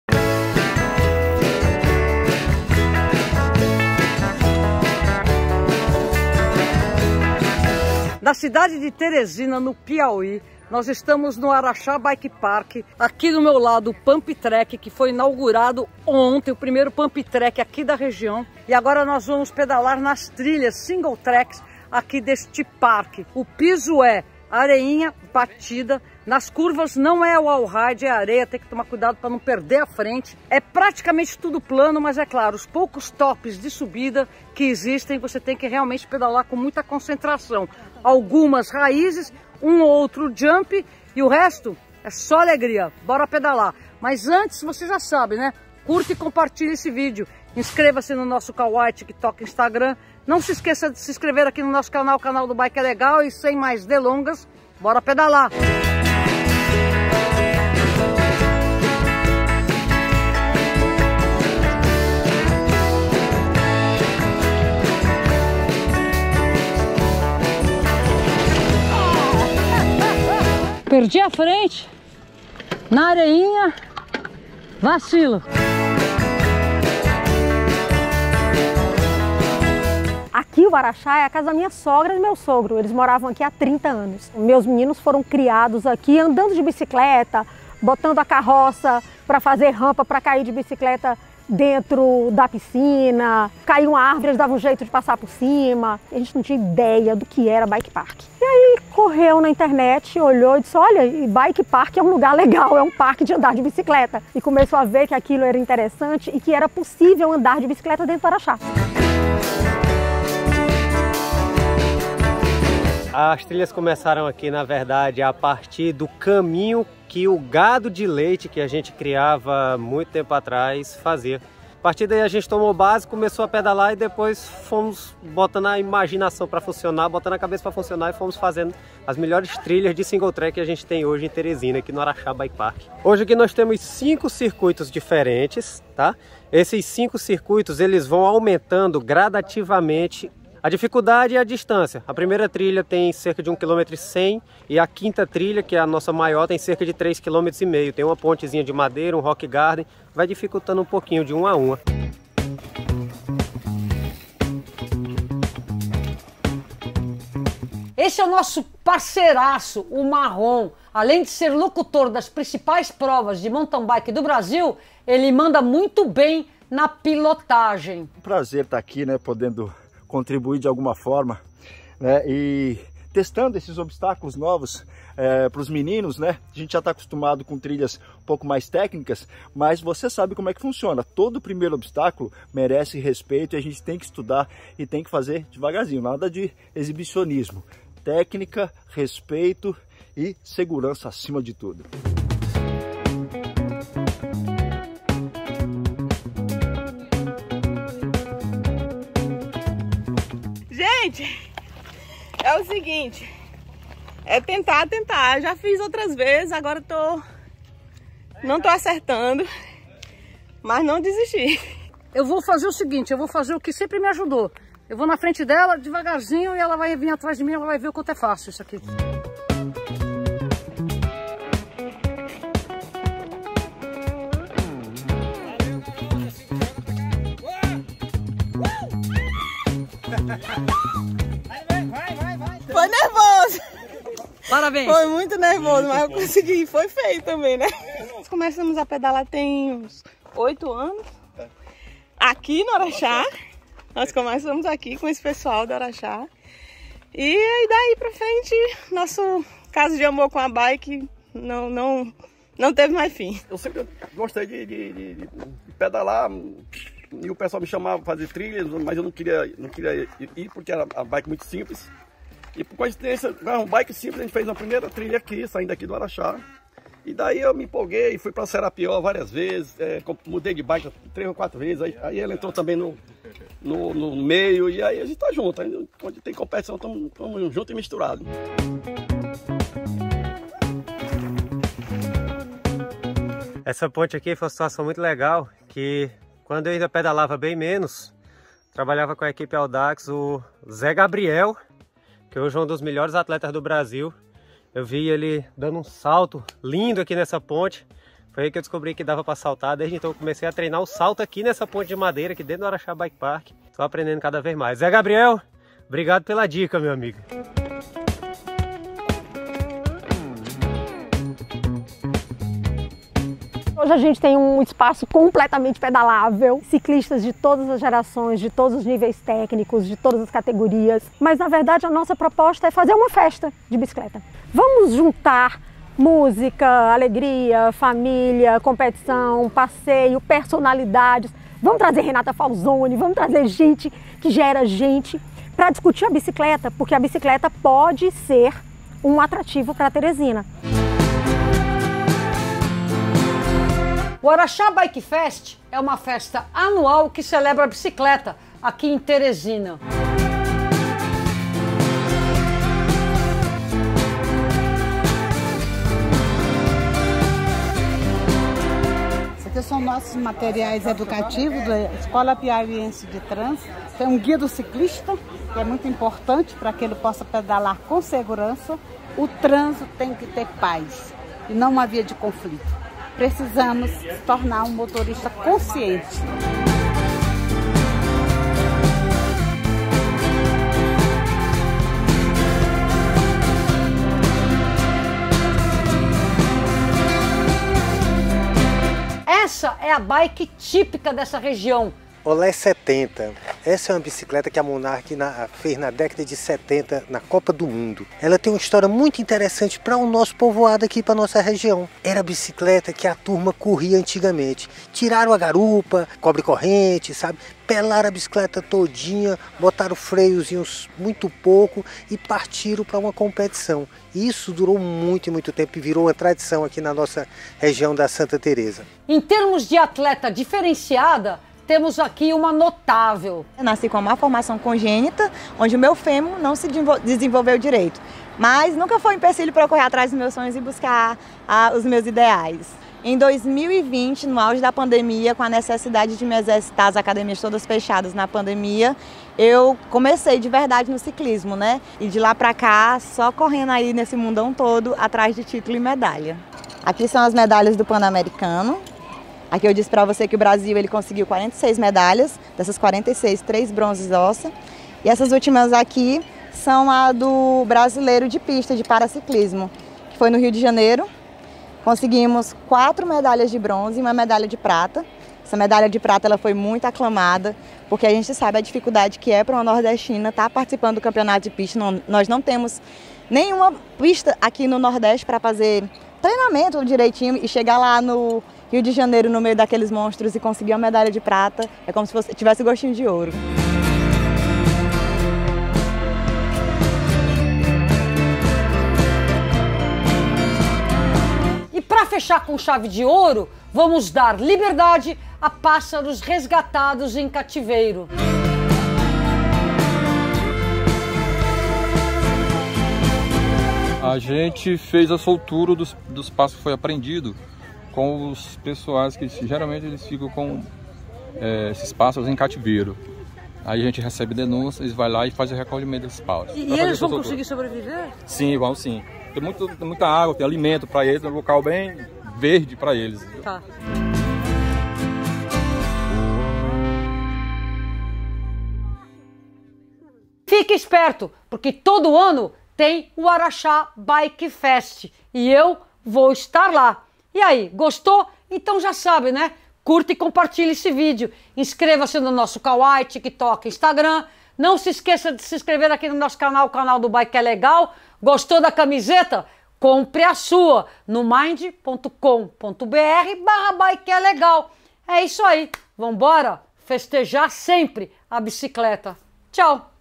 SÓ ALEGRIA NO BIKE PARK MAIS COMPLETO DO PIAUÍ!
Renata Falzoni conhece o Araxá Bike Park, um lugar completo para os amantes das bicicletas!

Acesse os link abaixo e adquira a sua camisa do Bike é Legal!